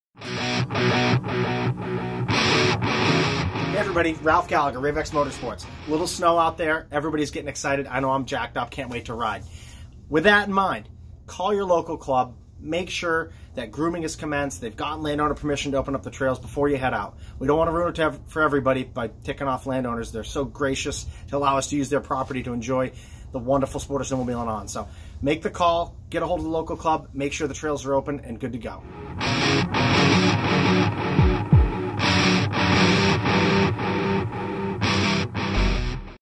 Know where to go! Maine backcountry PSA by Rave X
We know that you are all stoked to ride early but please check with clubs and landowners to make sure that you have early season riding permission.  Even if it is mid season, please check to make sure that the landowners are ok with it.  We are at risk of losing our great spots which will overpopulate other riding areas.  Help us prove to clubs and landowners that we can be responsible!  Join MARB (Maine Advocates for responsible Backcountry) today!